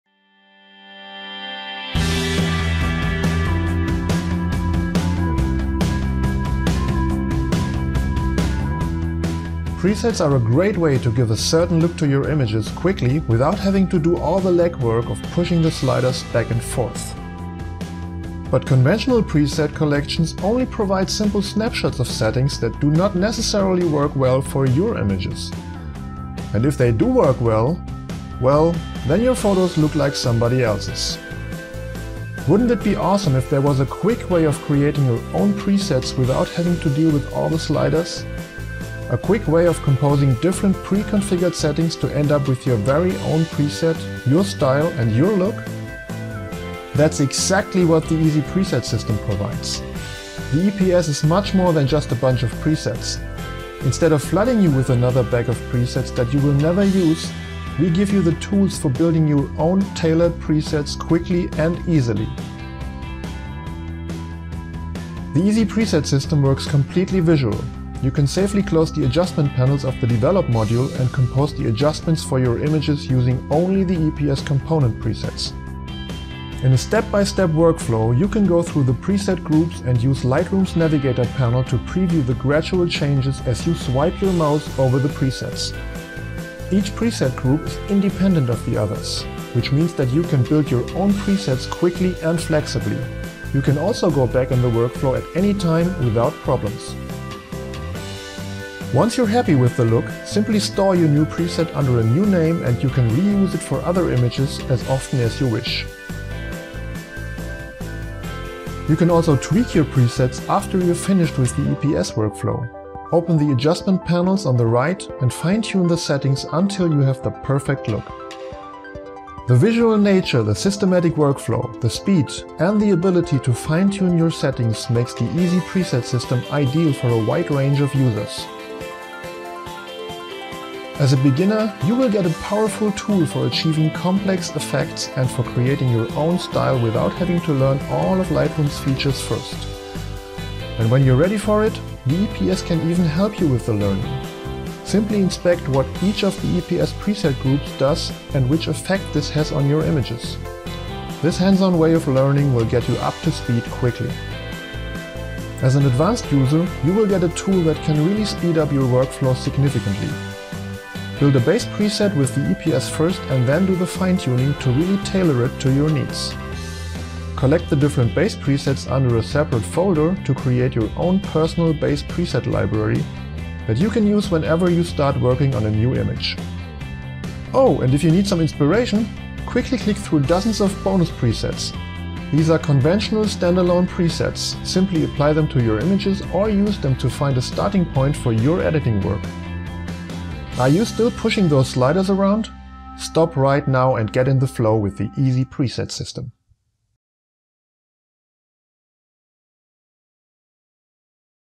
Easy Preset System for Lightroom - 01 - Introduction
The Easy Preset System (EPS) is a system for creating your own presets in a systematic and visual way. We have leveraged Lightroom's ability to stack presets, and we designed a series of component presets and a workflow that allows you to easily compose your adjustments, targeting each aspect of your image separately in a step-by-step manner. This process does not require you to touch any of the sliders in the Develop modul of Lightroom directly. You can compose the adjustments for an image quickly and easily and come up with a new preset.

In this video, I will give you an overview of the Easy Preset System for Lightroom. I will quickly go through all the features and show you how it works in principle.